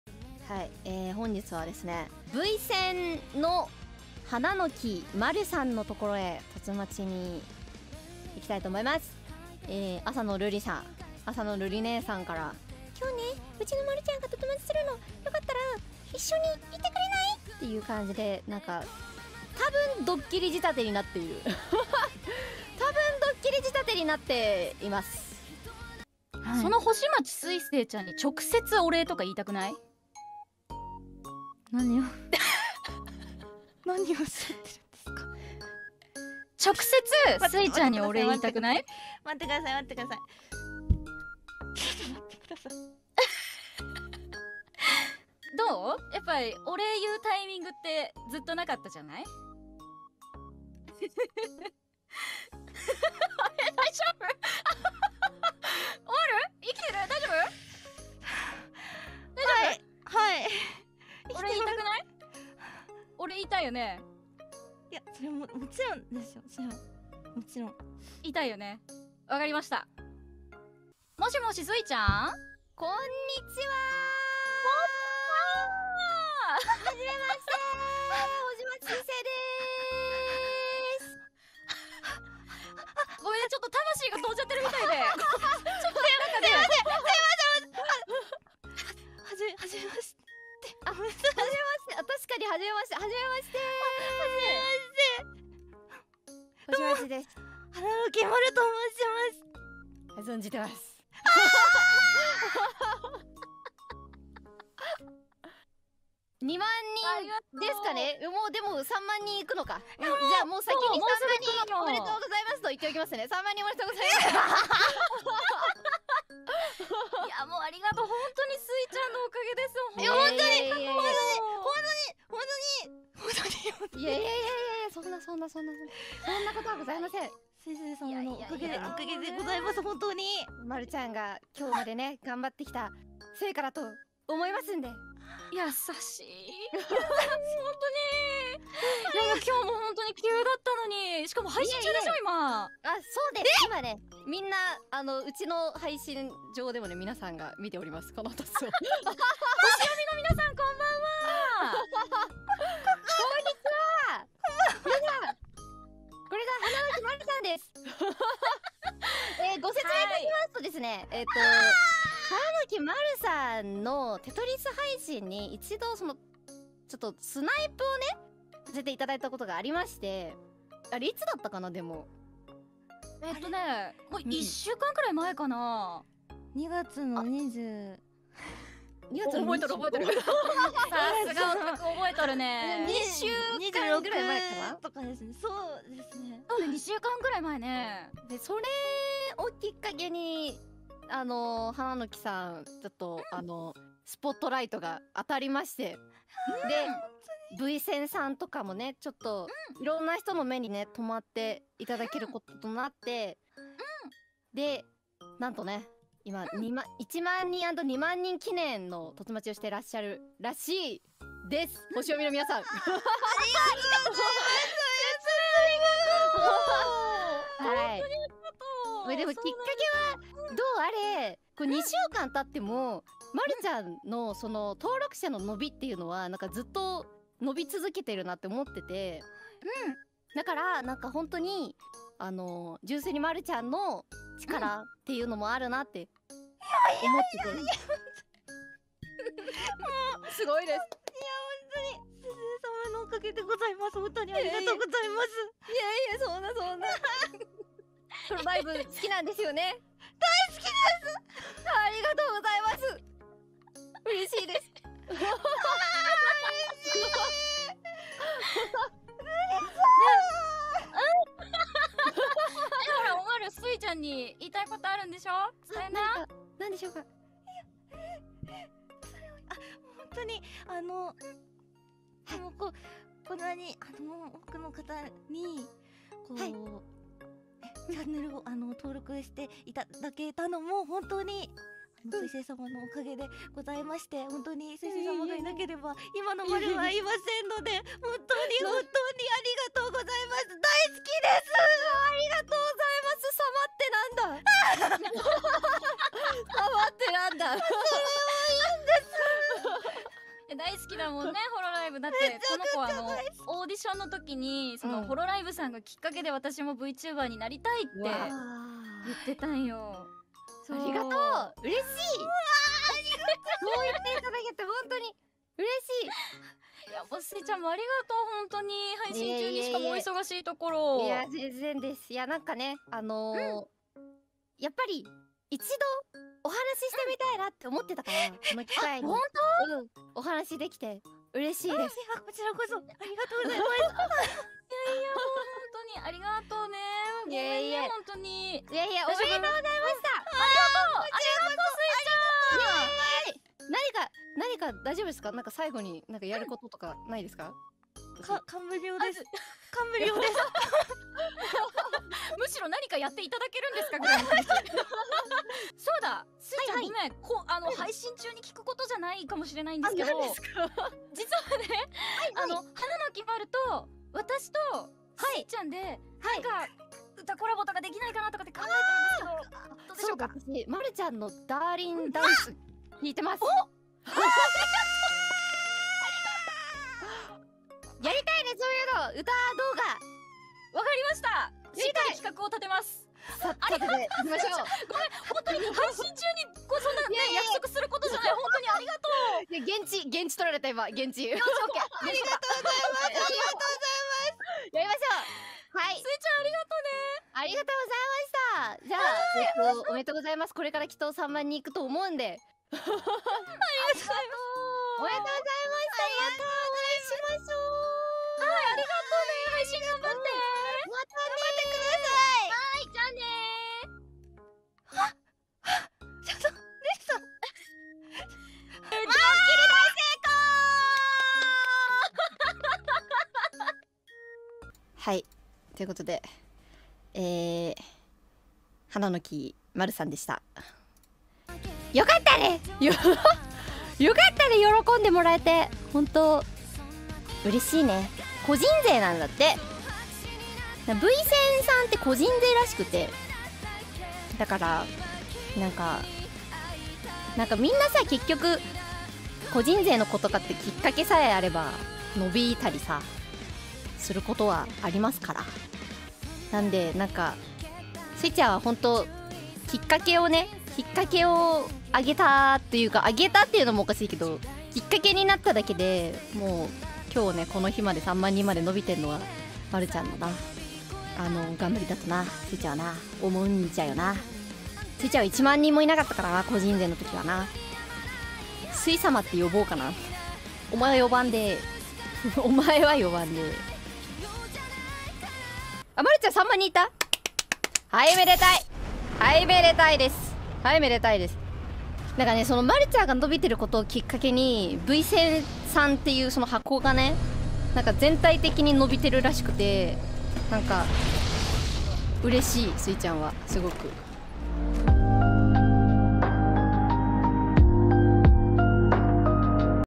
Hananoki Maru ascends once again as Suisei joins her call in stream【Hololive | Vsen | Eng Sub】
Congrats for 30k maru-channnn!!!
Do check out the reddit post made by Elc-the-Lad [エルちゃん]-san which provides more context towards the video!

〖Autumn〗

〖Spring〗

〖Summer〗

〖Winter〗